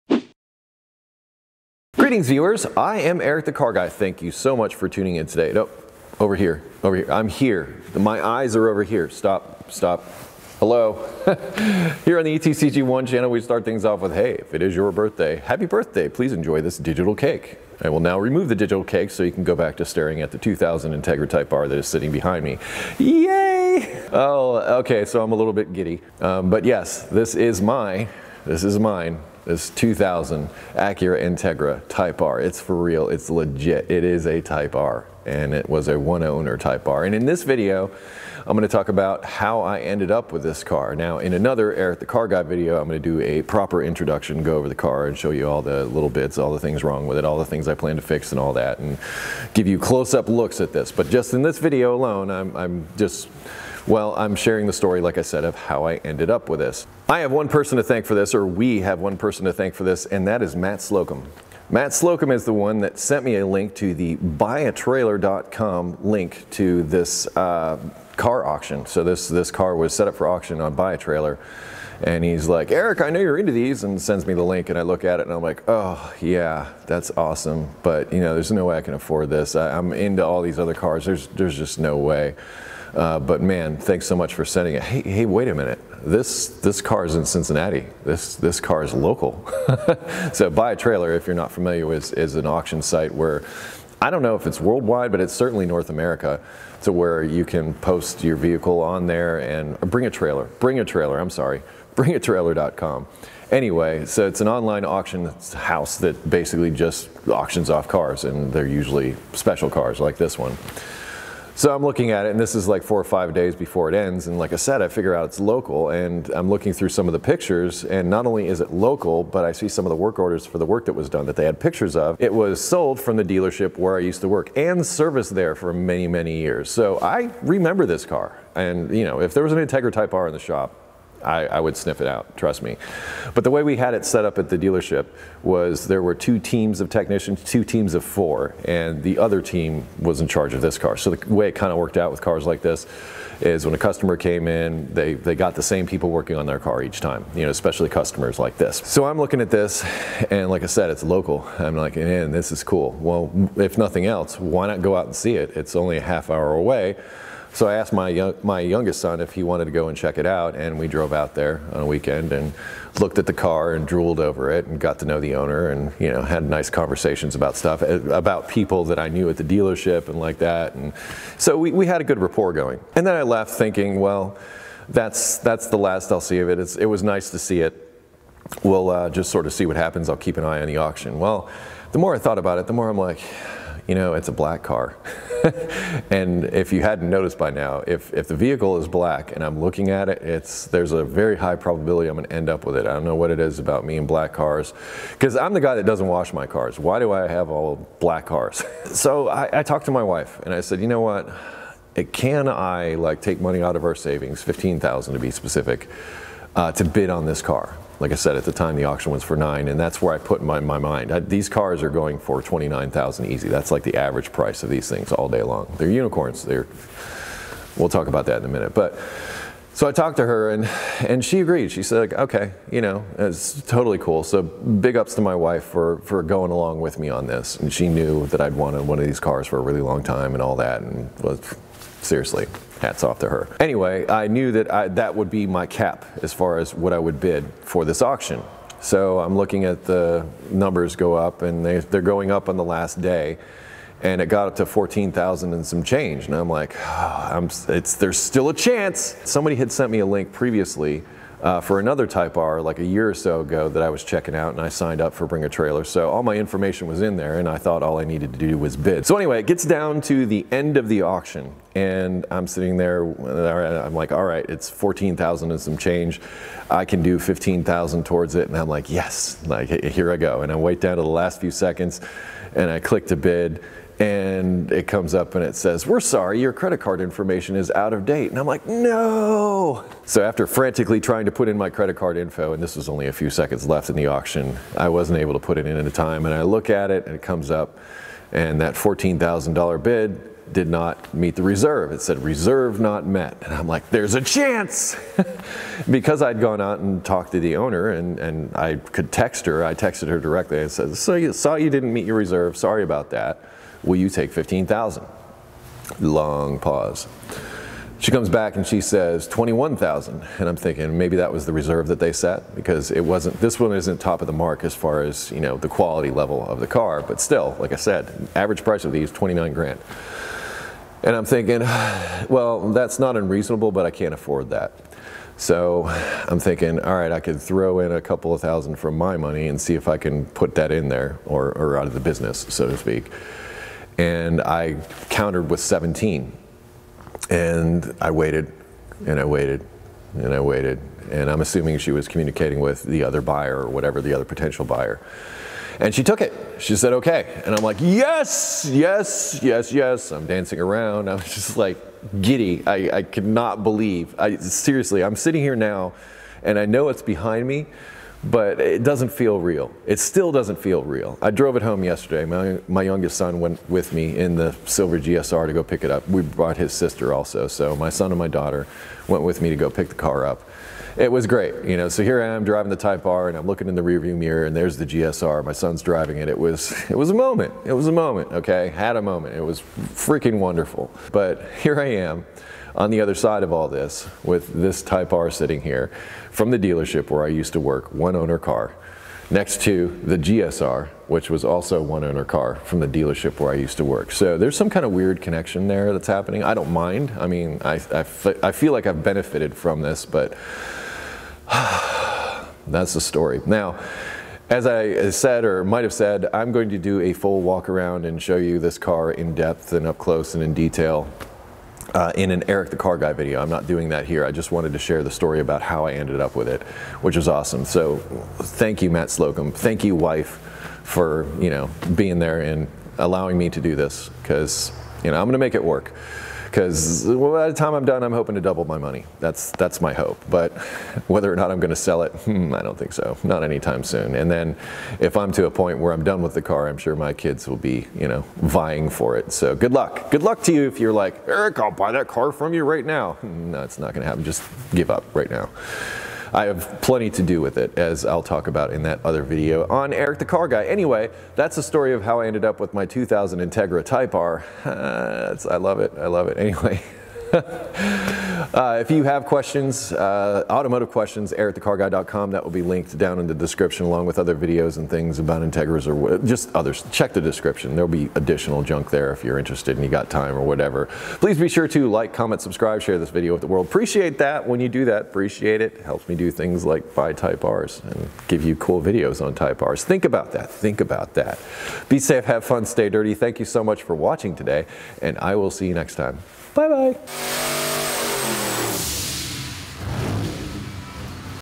How I Got My Type R
In this video I recount the roller coaster ride I took in getting this 2000 Integra Type R, which happens to be my dream car. Well, one of them anyway. As a “Car Guy”, I have a lot of dreams about cars.

Anyway, if you want to know how I got it, what I spent on it, and if I’m keeping the GSR, watch this video.

Links

Want more content from ETCG? Information on Premium Membership

Stay Dirty

ETCG1